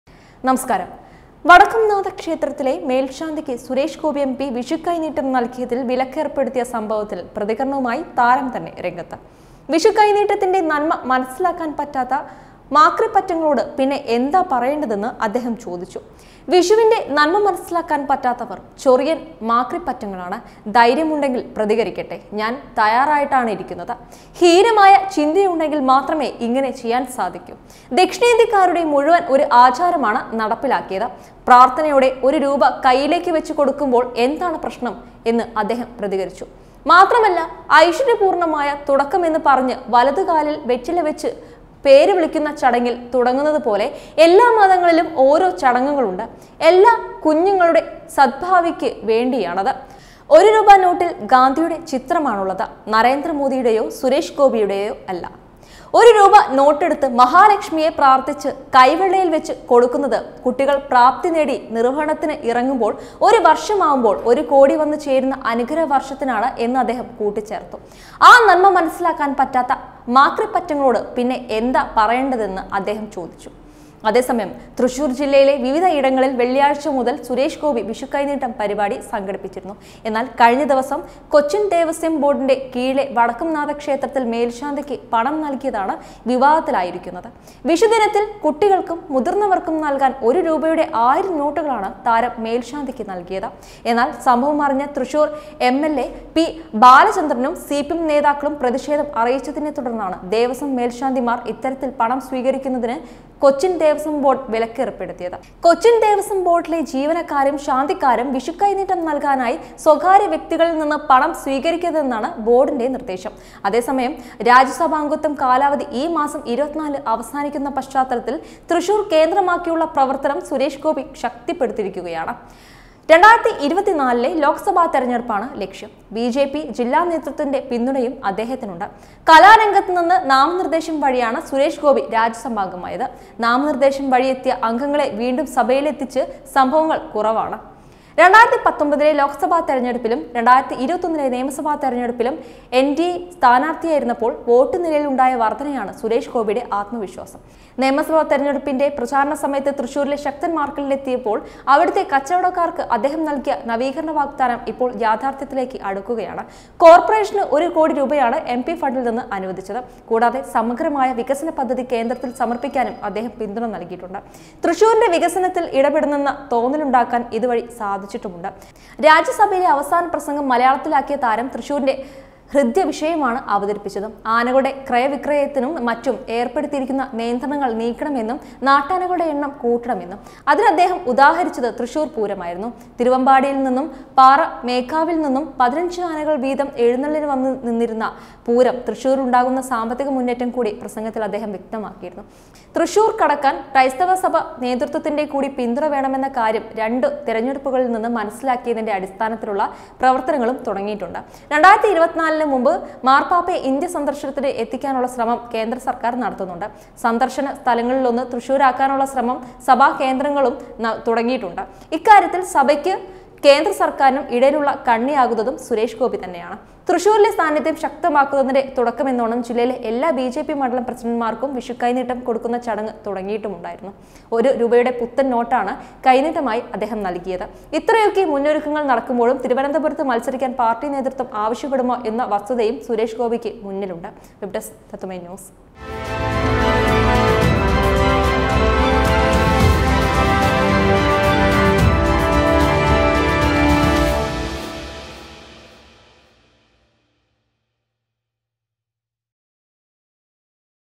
നൻമ മനസ്സിലാക്കാൻ പറ്റാത്ത മാക്രി പറ്റങ്ങളോട് എന്താണ് പറയാനുള്ളത്? പൊട്ടിത്തെറിച്ച് സുരേഷ് ഗോപി
നൻമ മനസ്സിലാക്കാൻ പറ്റാത്ത മാക്രി പറ്റങ്ങളോട് എന്താണ് പറയാനുള്ളത്? പൊട്ടിത്തെറിച്ച് സുരേഷ് ഗോപി |SURESH GOPI